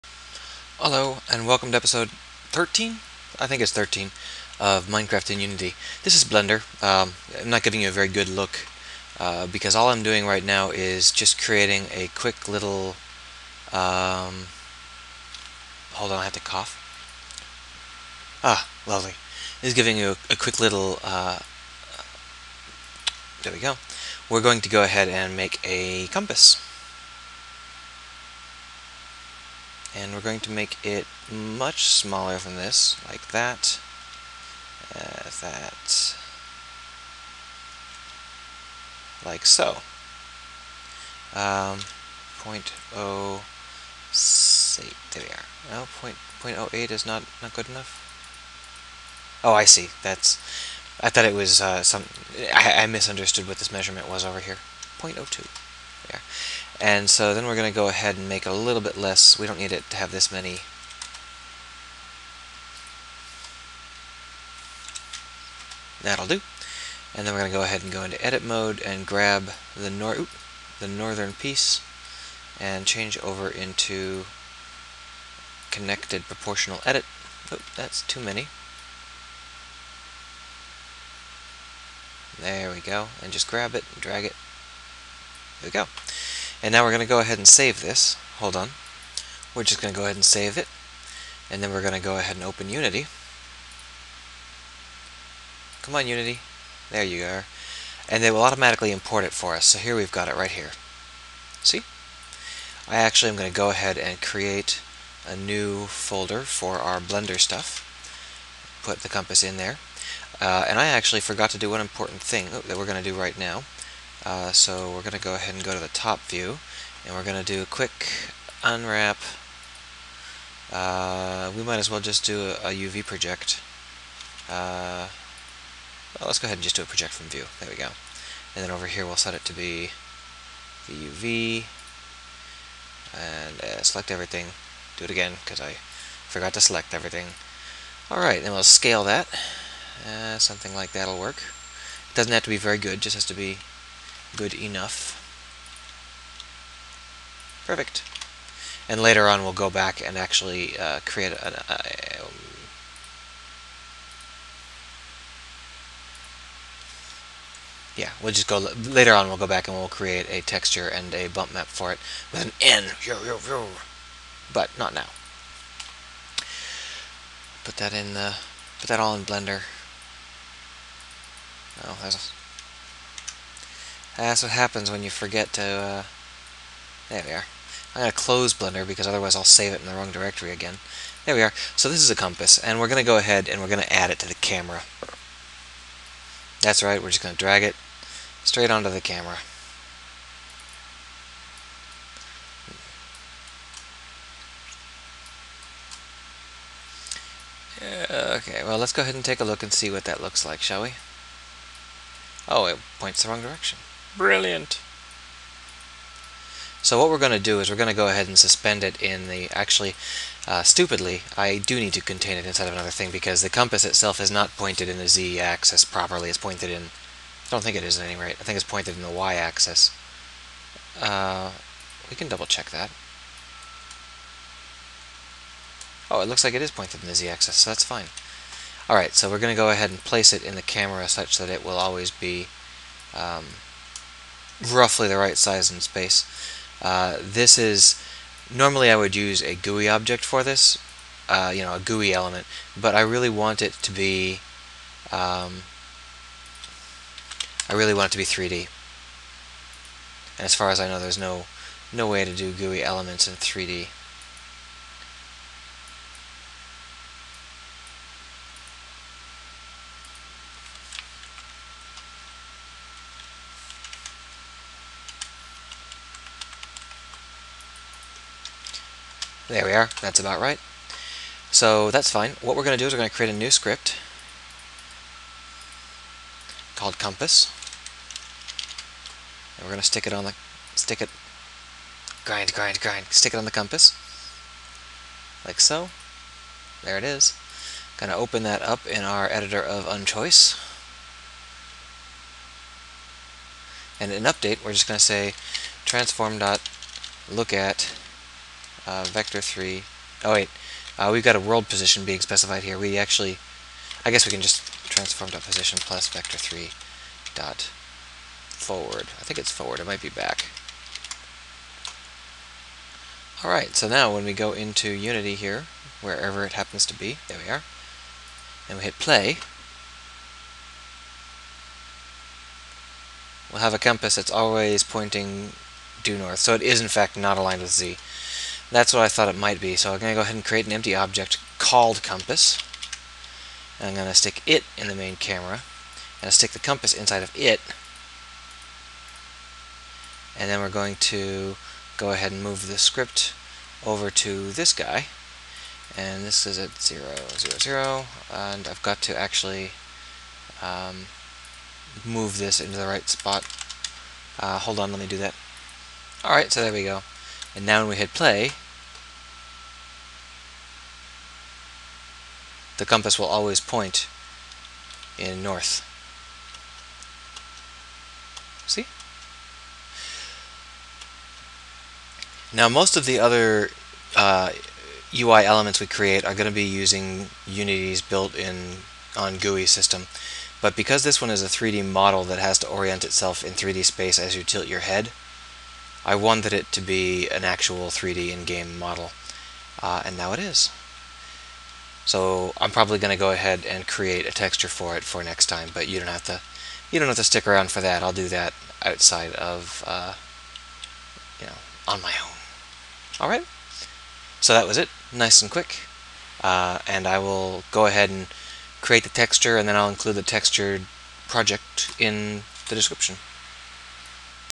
Tutorial: Minecraft in Unity #13 (Player's Compass)
You can grab the project with a slightly improved model and texture here. In this project, I've attached the compass to the player rather than the camera, meaning you don't see it unless you look down.

Also, various small bugfixes. Since I reimplemented a lot of this due to my time traveling, the code may look slightly different than in the early videos.